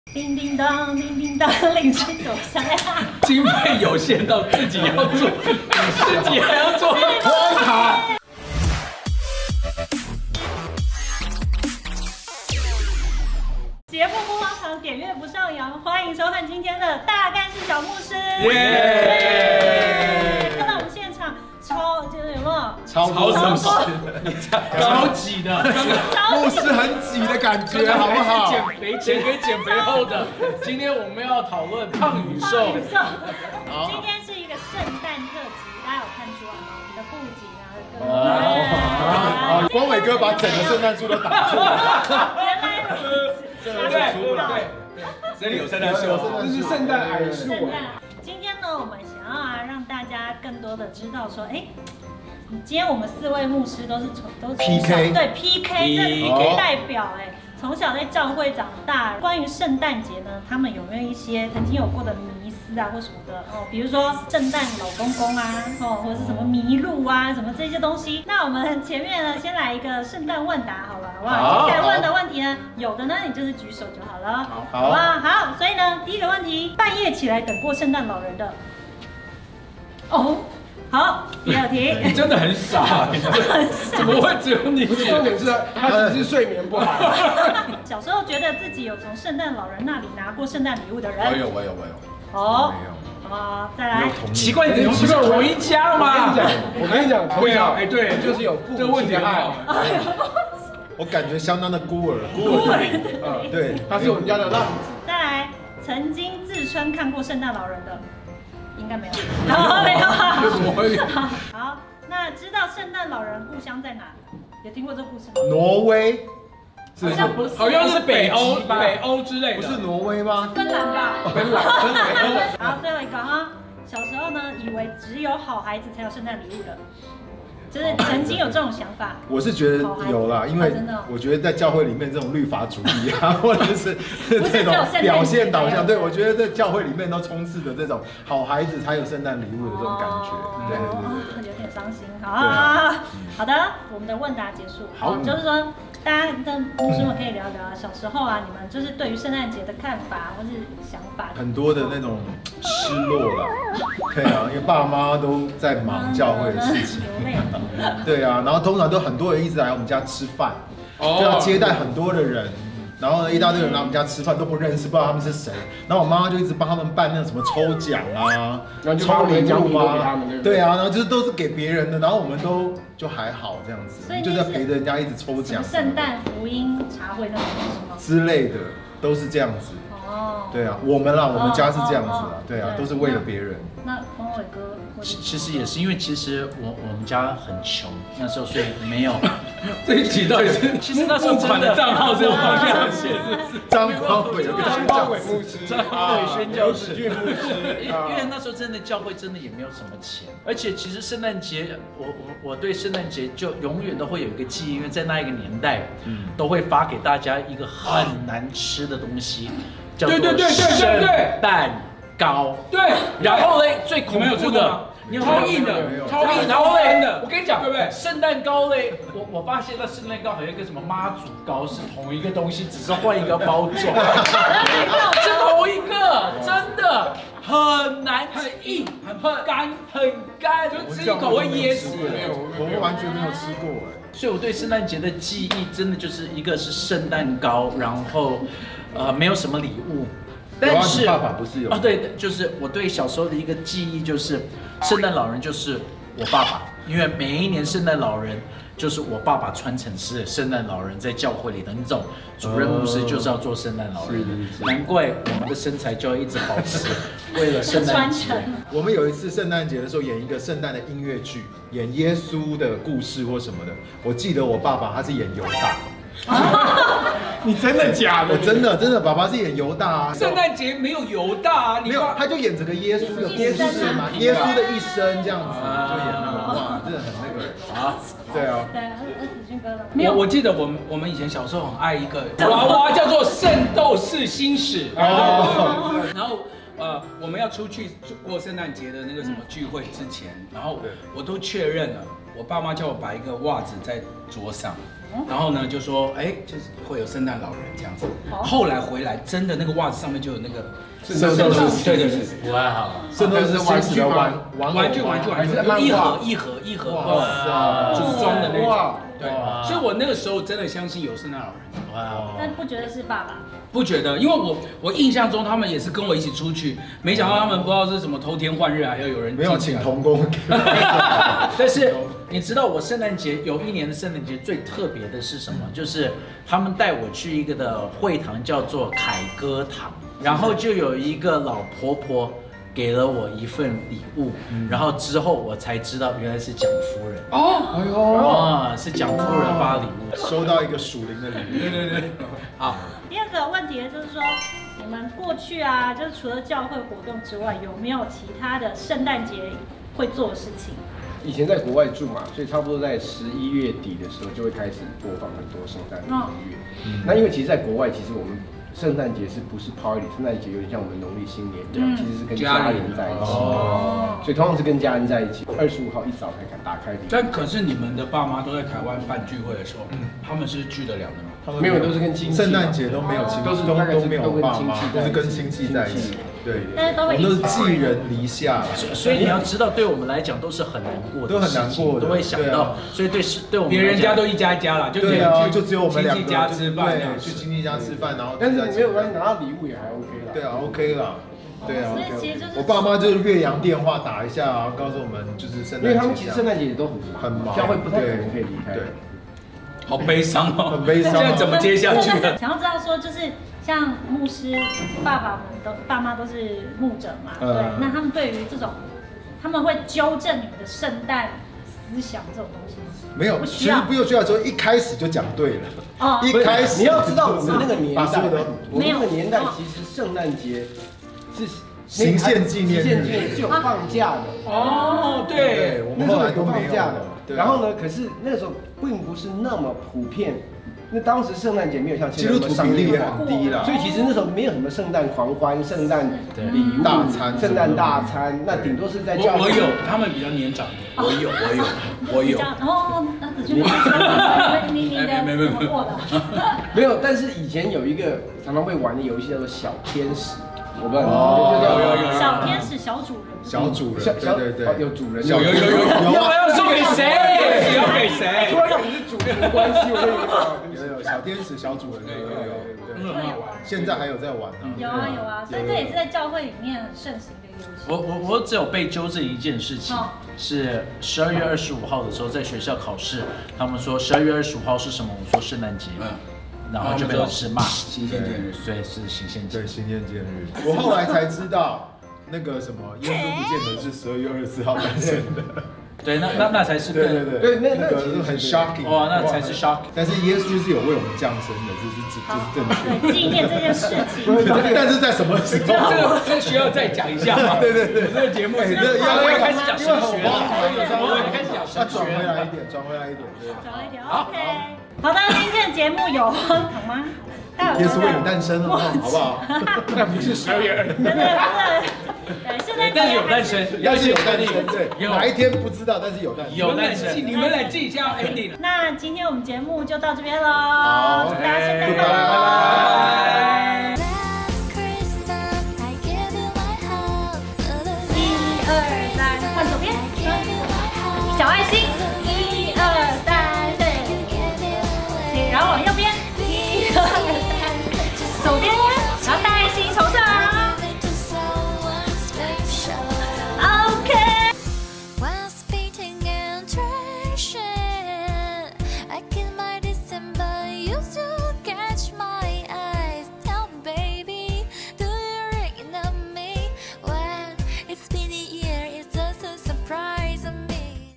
大幹事小牧師 ｜聖誕特輯-聖誕節是牧者們甜蜜的回憶還是淡淡的哀桑？？片尾有彩蛋（feat 子駿牧師、光偉牧師、巽正牧師）
聖誕節一定要甜甜的過啊？！千萬不要錯過今天的大幹事小牧師！！！

節目不荒唐 點閱不上揚
全新節目企劃，讓你認識不一樣的牧者和牧區，讓原本你以為難以觸碰的距離，進而拉近轉變你的眼球視角
每週二 18:00首播，敬請鎖定台北靈糧堂青年牧區youtube頻道

所有你要的資訊都在
⬇️下方的連結樹裡面喔